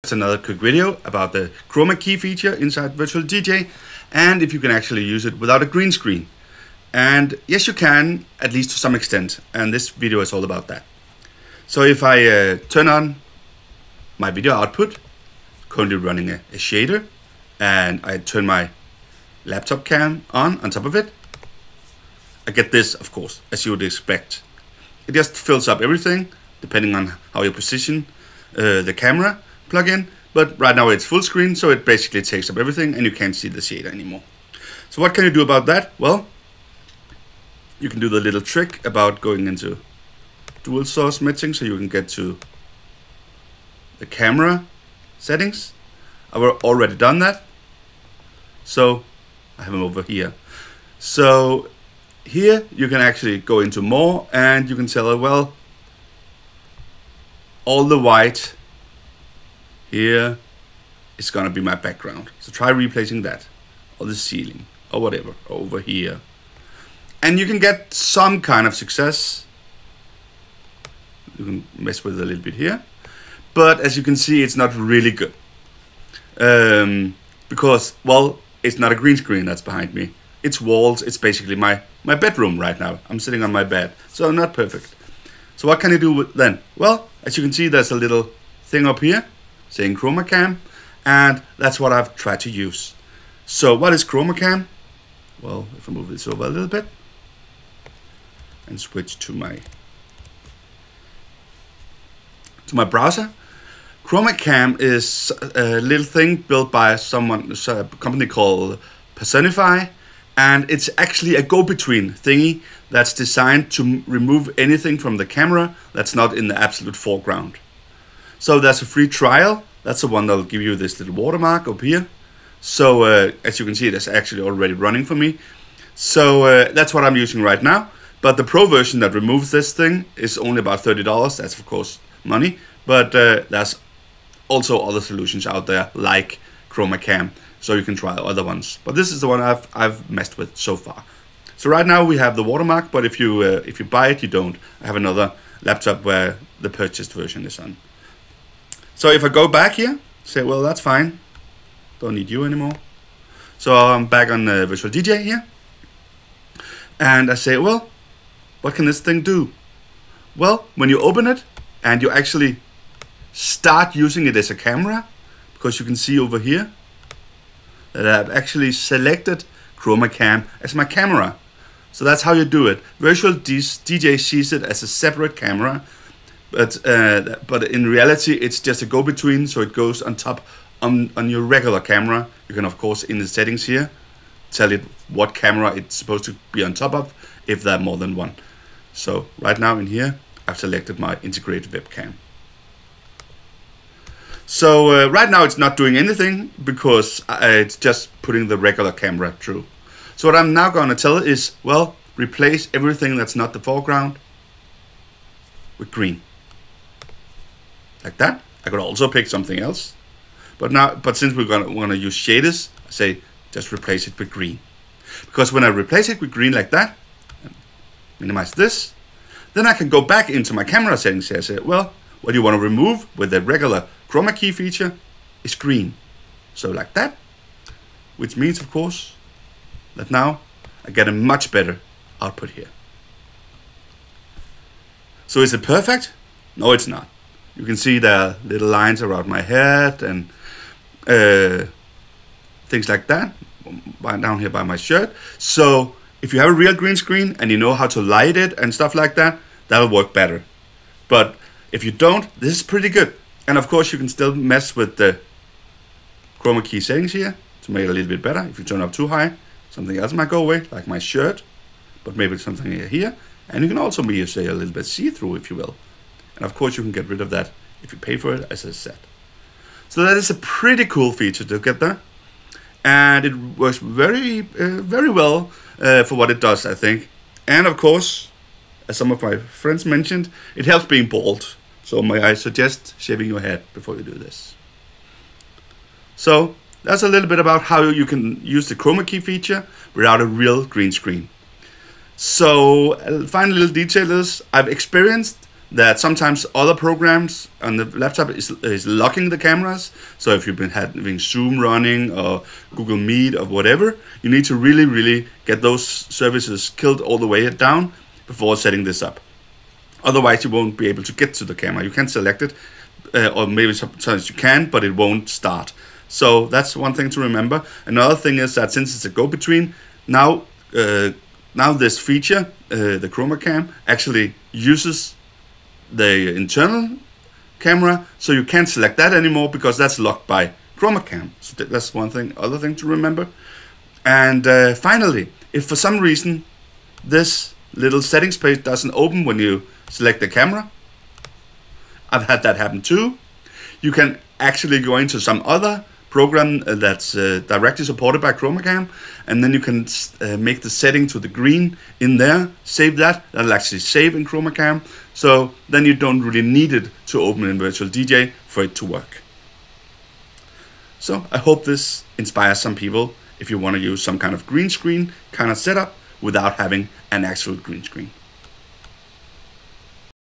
Quick n' Dirty: Using the Chroma Key feature without a green screen in VDJ 8.4/2020
This video shows how you can use the Chroma Key feature without a green screen in VDJ 8.4/2020 by using a "middle man" software

Thanks to Tom Coon (The Weather Dog) for the inspiration